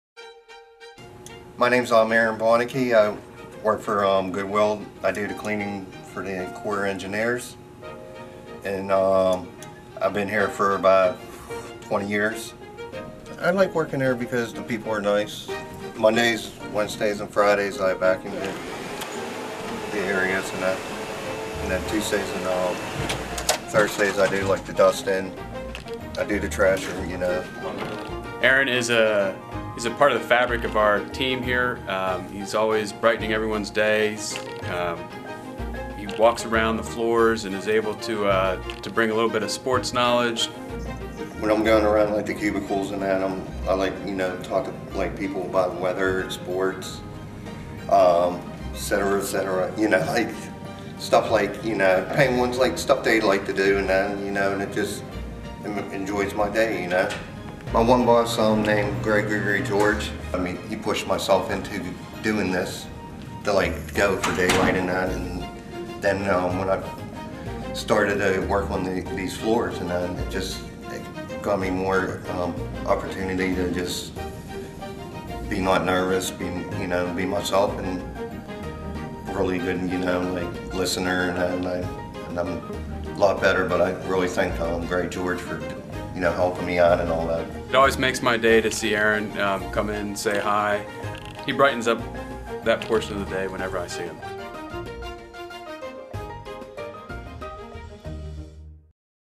Aaron Bonicky - Custodian
Today we highlight Aaron Bonicky, a custodian employed by Goodwill Service Industries for about 20 years. Not only does Aaron do a great job; but he also brightens everyone's day - keeping us informed on all the Pittsburgh sports news. Thank you, Aaron for all you do.

In the comments, tell us what you like to talk about with Aaron. Is it music, The Penguins or Steelers? Let us know. And as always, please 'Like" and "Share" our page. Thanks!